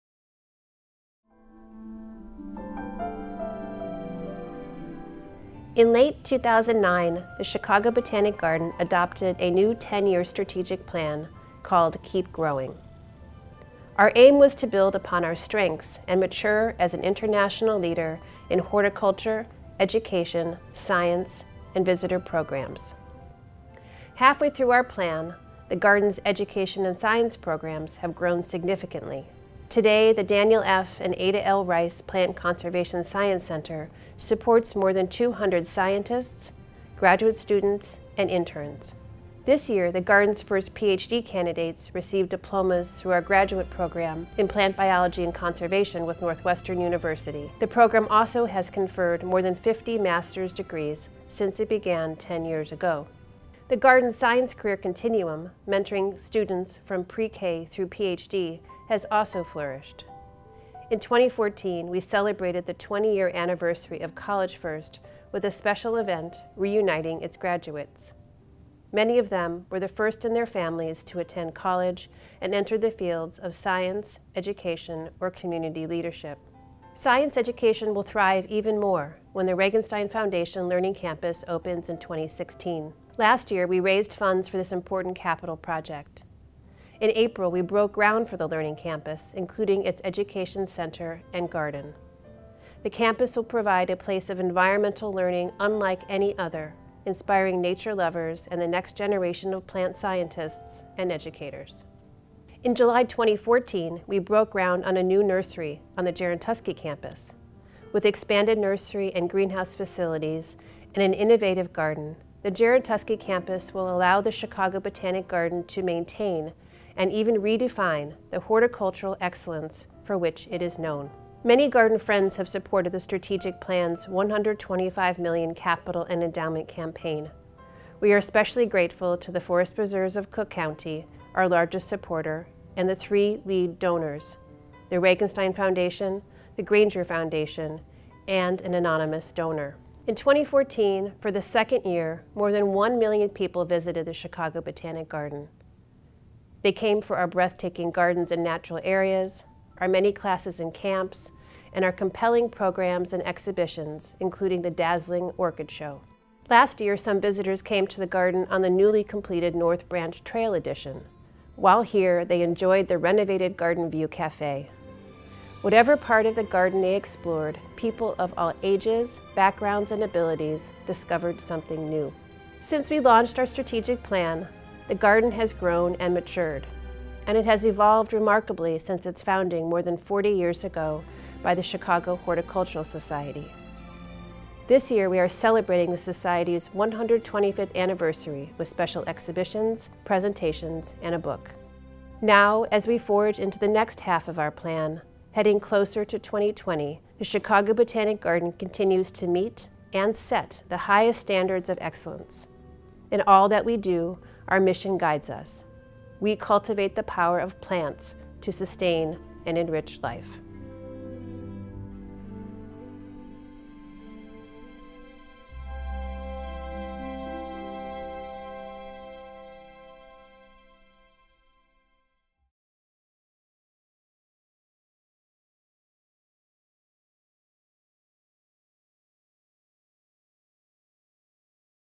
Chicago Botanic Garden Annual Report 2014 - with Sophia Shaw and Robert Finke
"Keep Growing" Year Five: Our accomplishments and plans as envisioned in our 10-year Strategic Plan. We invite you to view our strategic plan for more information.